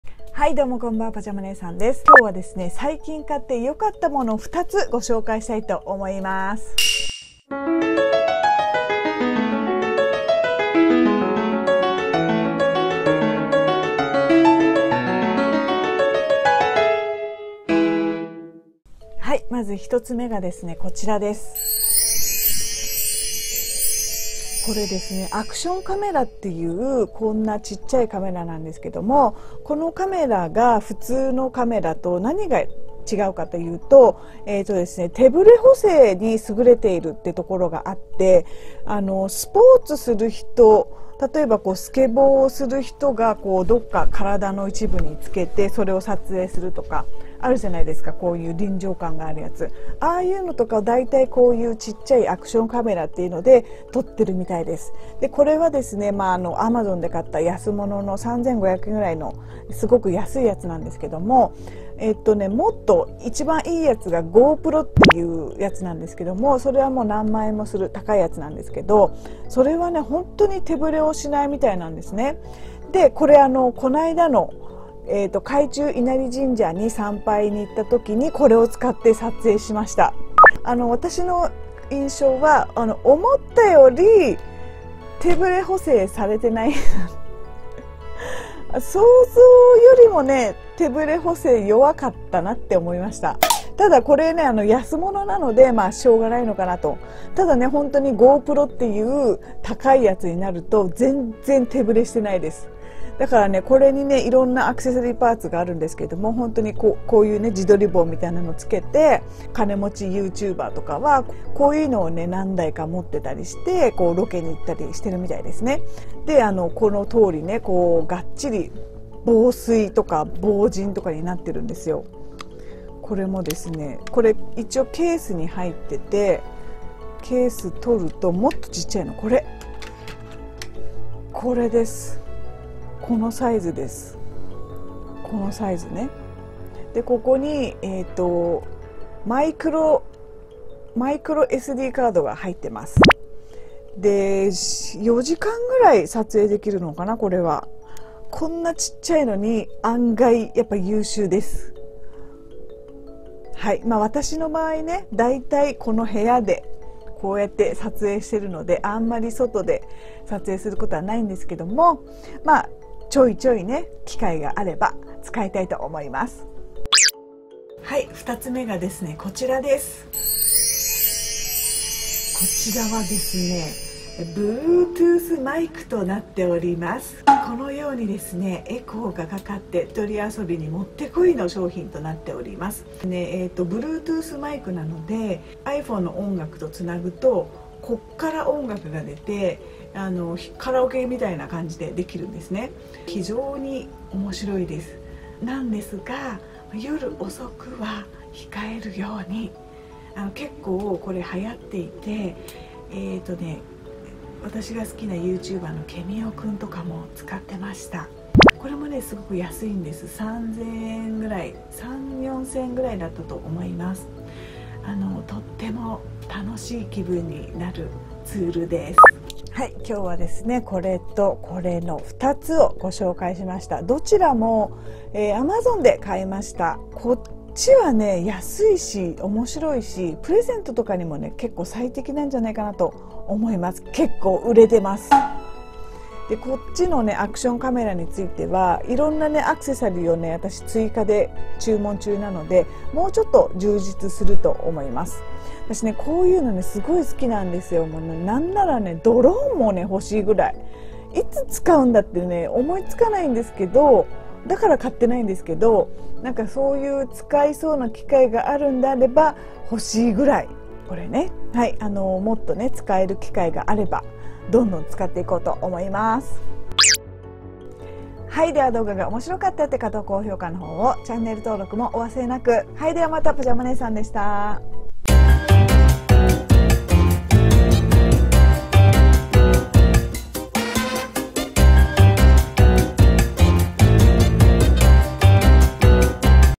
【Vlog】最近買ってよかったものご紹介！Action Camera / Bluetooth マイク
Amazonヘビーユーザーのパジャマ姉さんですｗ

◆Action Camera アクションカメラはこちら

◆Bluetooth マイクはこちら

◆パジャマ姉さん　Instagram